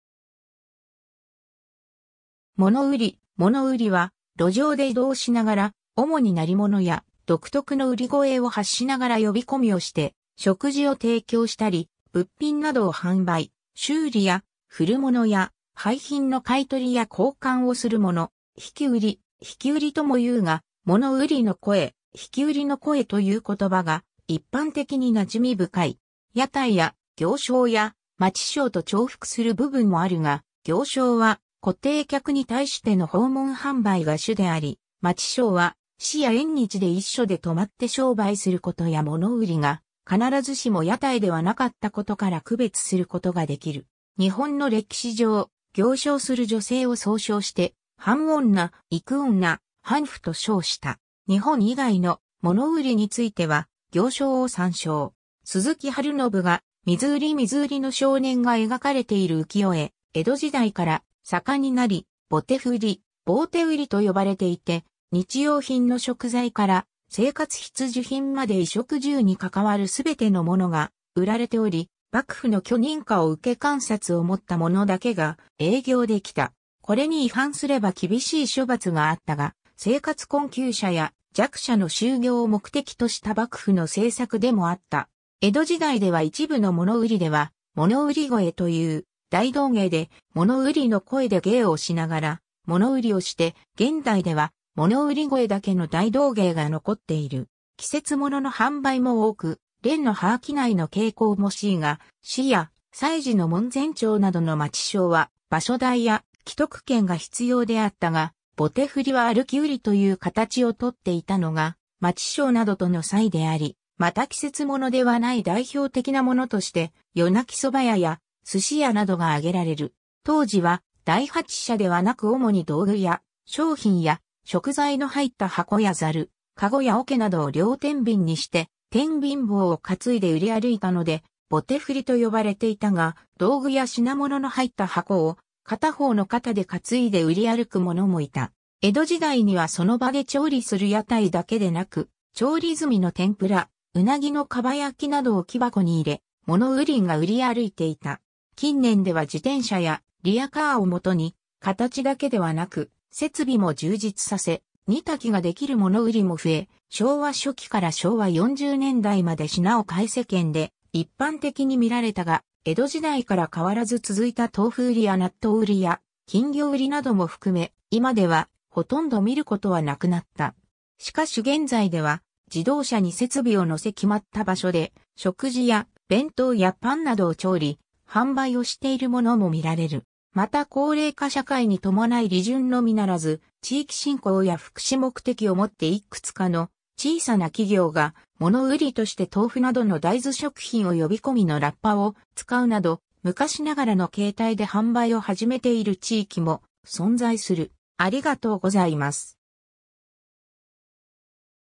物売り
商制度・商習慣
物売、物売り（ものうり）は、路上で移動しながら、主に鳴り物や独特の売り声を発しながら呼び込みをして食事を提供したり、物品などを販売、修理や古物や廃品の買取や交換をする者。
「引き売り」・「引売り」ともいうが「物売りの声」・「引き売りの声」という言葉が一般的に馴染深い。
屋台や行商や街商と重複する部分もあるが、行商は固定客に対しての訪問販売が主であり、街商は市や縁日で一所（ひとつところ）で留まって商売する事や物売りが必ずしも屋台ではなかった事から区別する事が出来る。
日本の歴史上、行商する女性を総称して、販女、鬻女、販婦（ひさぎめ）と称した。
 日本以外の「物売」については、行商を参照。
 鈴木春信画：『水売り』水売りの少年が描かれている浮世絵 江戸時代から盛んになり「棒手振」・「棒手売」（ぼてふり）と呼ばれていて日用品の食材から生活必需品まで衣食住に係わる全てのものが売られており幕府の許認可を受け鑑札を持った物だけが営業できた。
これに違反すれば厳しい処罰があったが、生活困窮者や弱者の就業を目的とした幕府の政策でもあった。
 江戸時代では一部の物売りでは物売り声と言う大道芸で物売りの声で芸をしながら、物売りをして、現代では物売り声だけの大道芸が残っている。
 季節物の販売も多く、蓮の葉商いの傾向も強いが市や祭事の門前町などの街商は場所代や既得権が必要であったが、棒手振は歩き売りという形を取っていたのが街商などとの差異であり、また季節物ではない代表的なものとして、夜鳴き蕎麦屋や鮨屋などがあげられる。
当時は大八車（リヤカー）ではなく主に道具や商品や食材の入った箱や笊（ざる）、籠（かご）や桶（おけ）などを両天秤にして天秤棒を担いで売り歩いたので棒手振と呼ばれていたが、道具や品物の入った箱を片方の肩で担いで売り歩く者もいた。
 江戸時代にはその場で調理する屋台だけでなく、調理済みの天麩羅（てんぷら）、鰻（うなぎ）の蒲焼などを木箱に入れ、物売りが売り歩いていた。
近年では自転車やリヤカーをもとに形だけではなく設備も充実させ、煮炊きが出来る（ただし江戸時代から屋台では煮炊きの設備を備えていた）物売りも増え、昭和初期から昭和40年代まで品を替え（鮨や蕎麦は廃れおでん売りや菓子パン売りなどが盛んになった）世間で一般的に見られたが、江戸時代から変わらず続いた豆腐売りや納豆売りや金魚売りなども含め、今ではほとんど見る事はなくなった。
 しかし現在では自動車に設備を載せ決まった場所（就労者は多いが外食産業や商店の無い所など）で食事や弁当やパンなどを調理、販売をしているものも見られる。
また高齢化社会に伴い利潤のみならず地域振興や福祉目的を持って幾つかの小さな企業が、物売りとして豆腐などの大豆食品を呼び込みのラッパを使うなど、昔ながらの形態で販売を始めている地域も存在する。